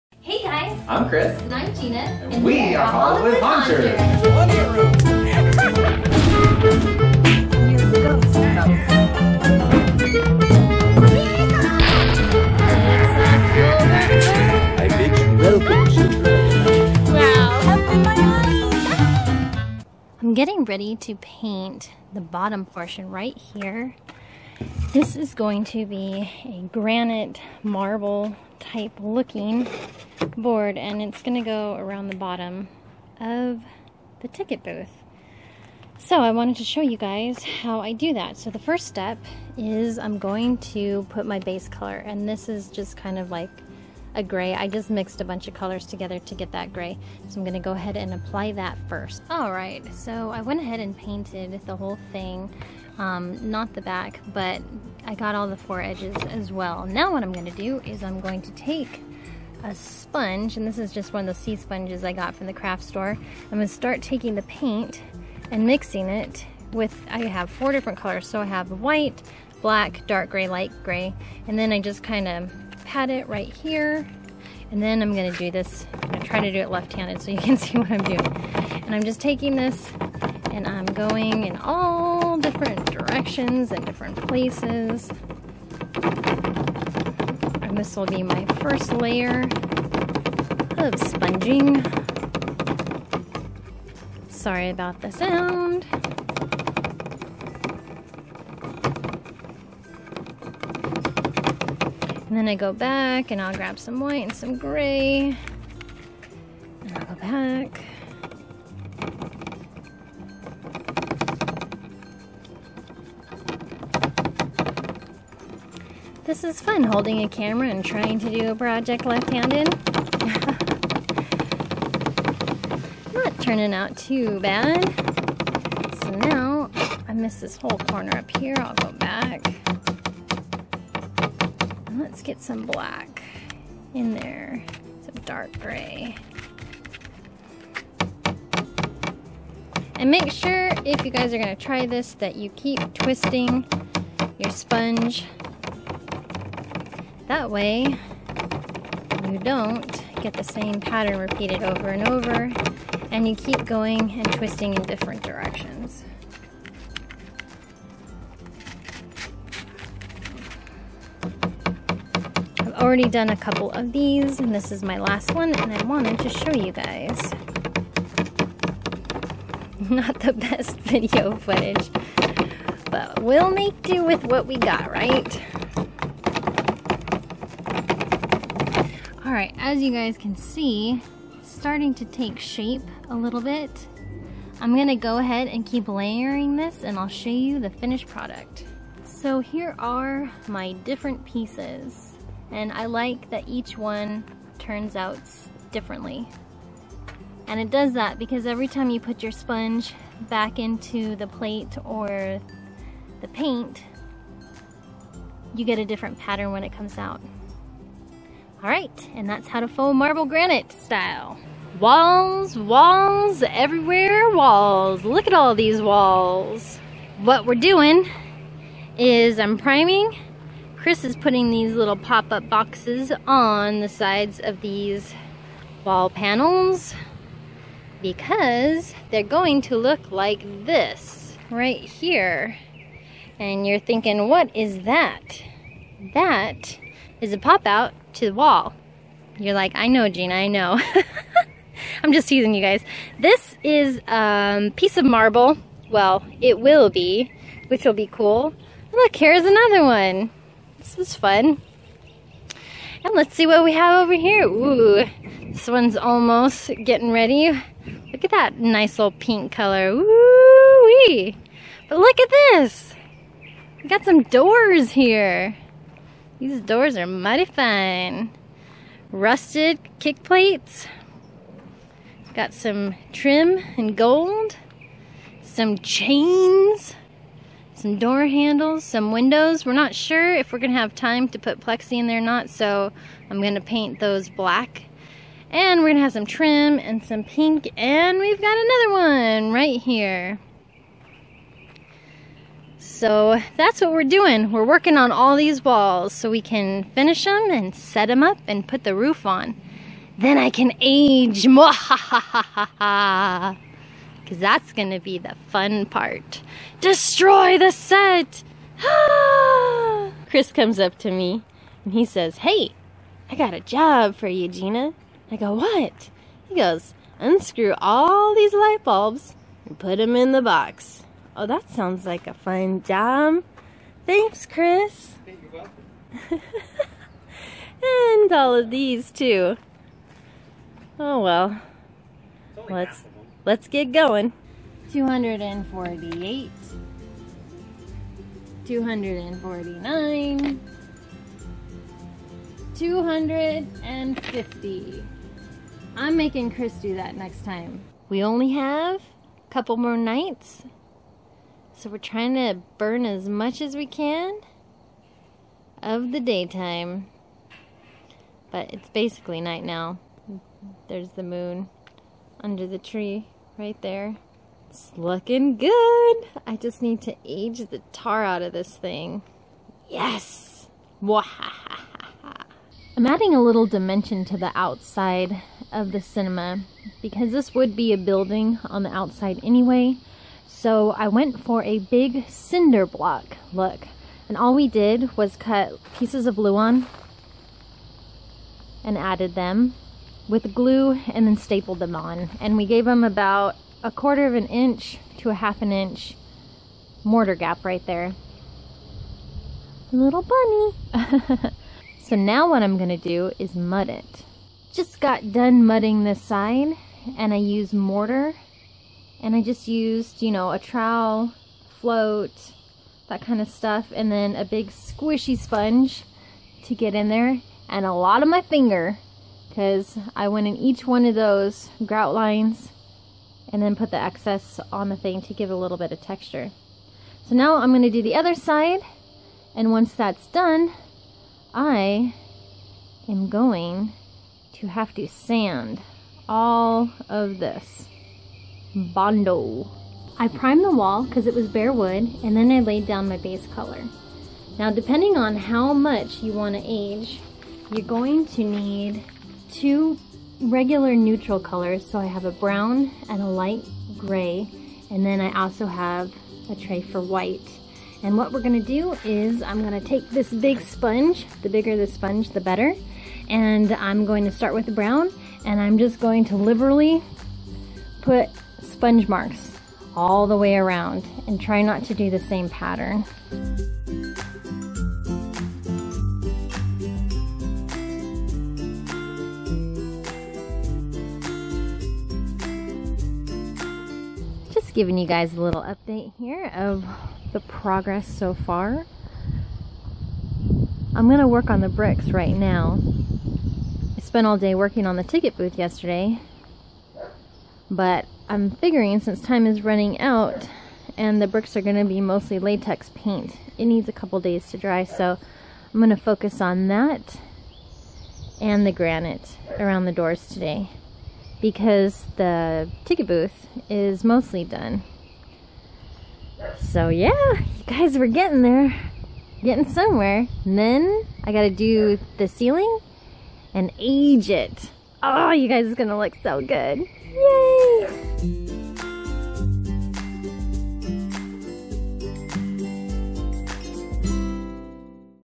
Halloween Haunted Movie Theater Facade (Pt.9) Faux Finish Paint Techniques For The Theater
Make an old abandoned cinema Halloween facade.  How to build and paint a movie theater facade series.  Join us as we begin a new Halloween Home Haunting project for The West Coast Haunters Convention.  We are constructing a trade show booth that will look like an old 1920's movie theatre marquee entrance.  The front of this haunted cinema will look run down, old, and abandoned.  We are designing the set as we go.  Each day we work on the scenery together we will add as much detail and we can think up and vlog as much as we can.  We hope to share some of the creative process in the making of this walk through trade show booth.  We hope you join us for this fun project and who knows we may learn some new ideas and techniques.  In this video we start to paint the ticket booth, walls, and ceiling of the old haunted movie theater.  In this Making An Old Abandoned Movie Theater Part Nine we paint the false double doors to the theater facade.  We share how we faux marble finish and also start to paint a damaged and old plaster wall look to the hollywood flats.

If you would like to see many more prop making videos check out our YouTube Channel